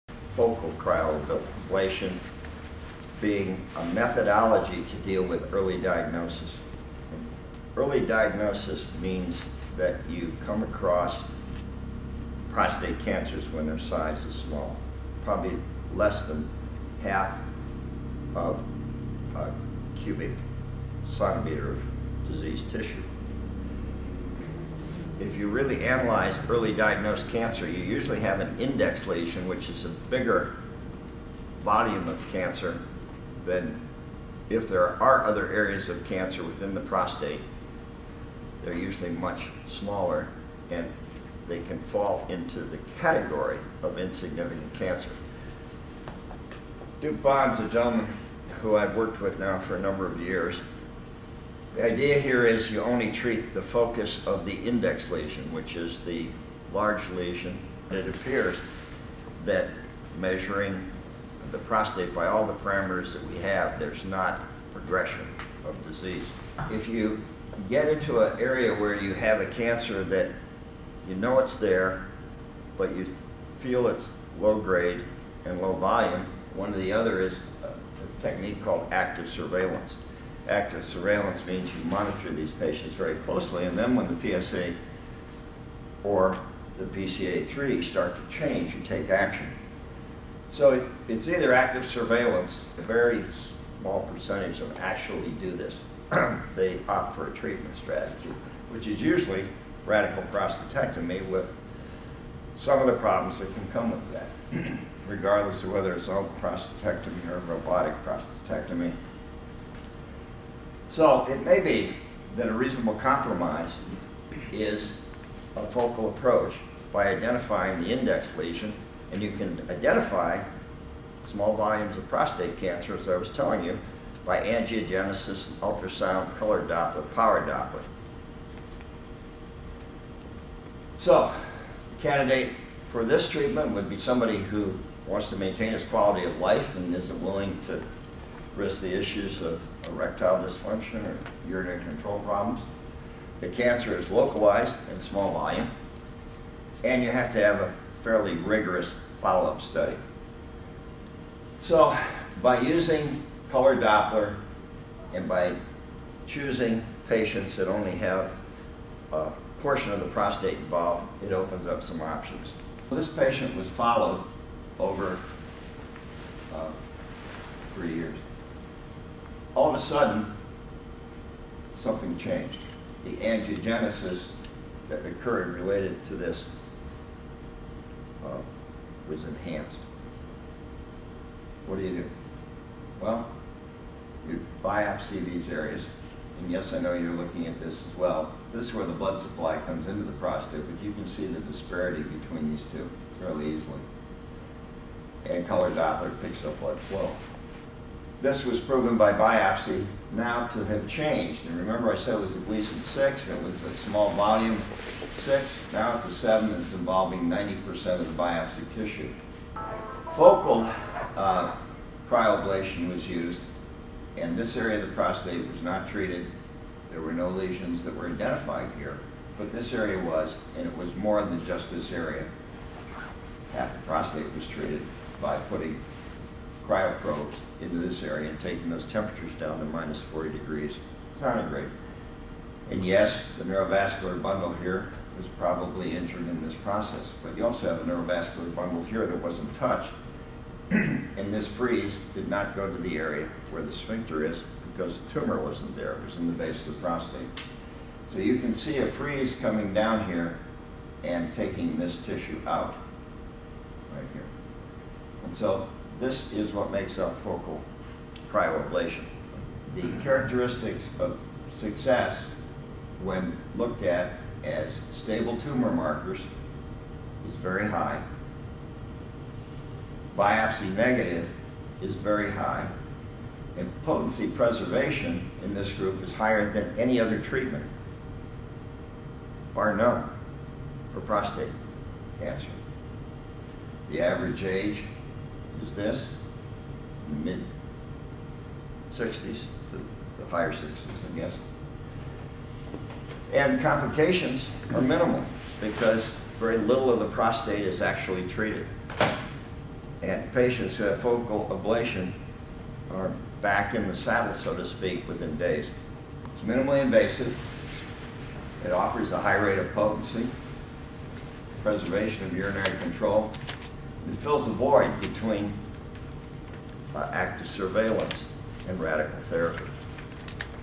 Focal Cryoablation of the Prostate urology601
This is a short clip by Dr. Herb Riemenschneider of Riverside Urology in Columbus, Ohio about Focal Cryoablation of the Prostate. What is it and when is it an appropriate approach for prostate cancer?